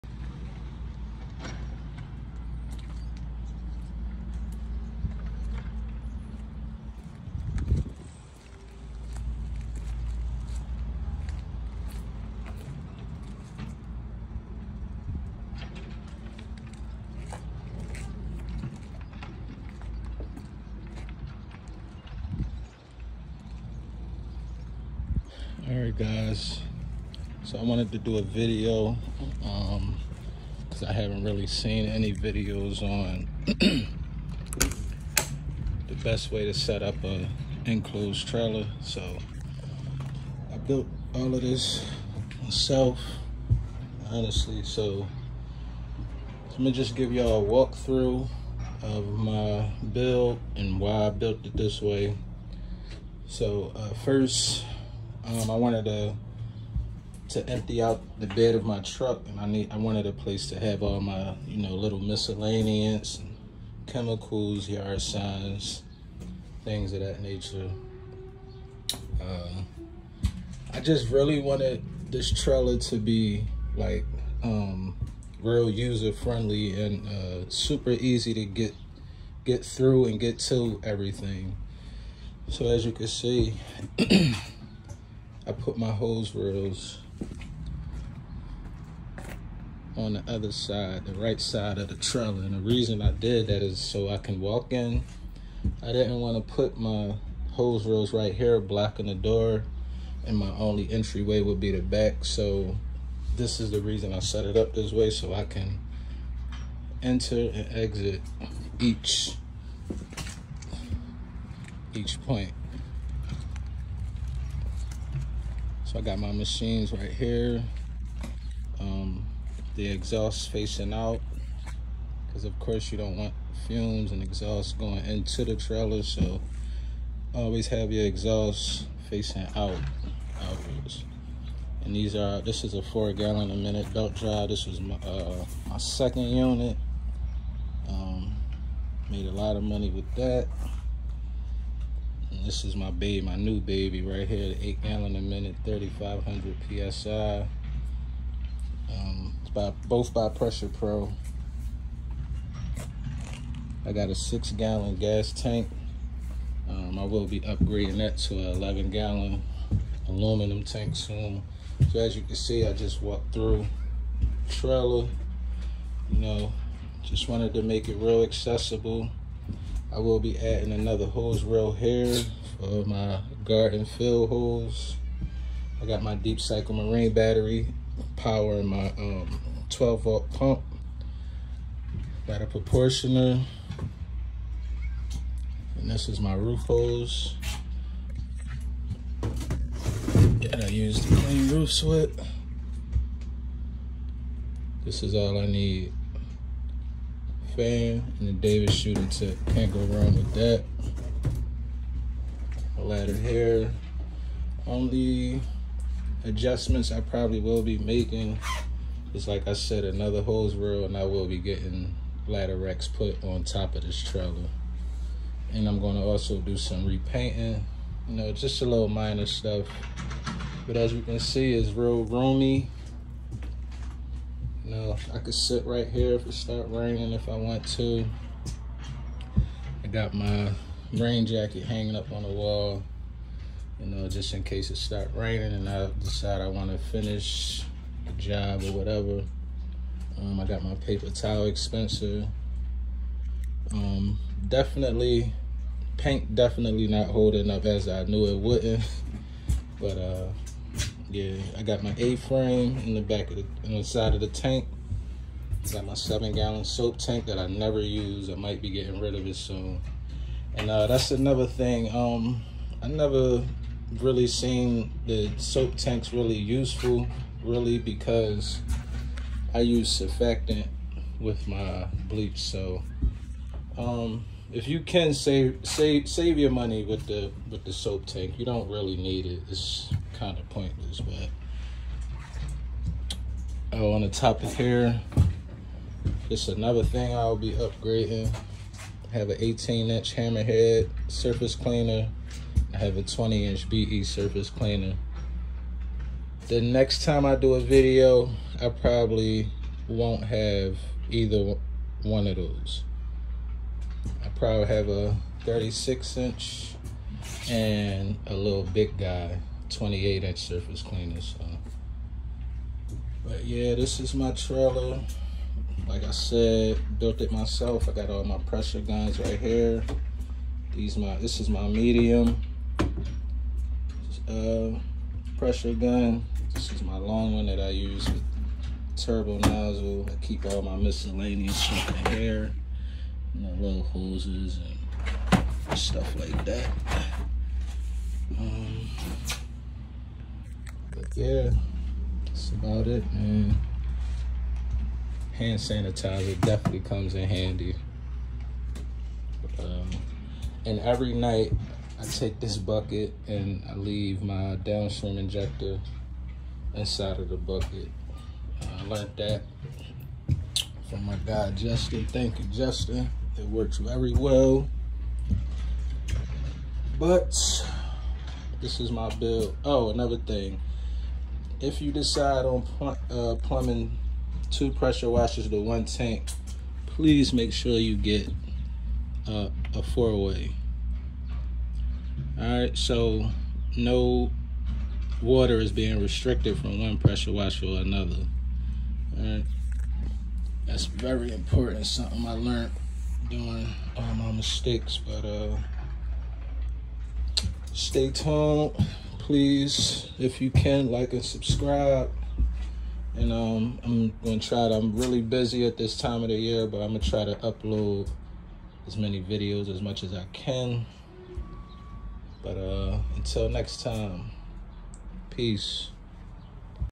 Overview: 7 X 14 enclosed pressure washing trailer.
8gpm Pressure Washer
4gpm Pressure Washer
275 gallon water tank
55 gallon chemical tank
7 gallon soap tank
2 titan hose reels (both 250 feet of pressure hose)
1 hardcore hose reel (200 feet soft wash hose)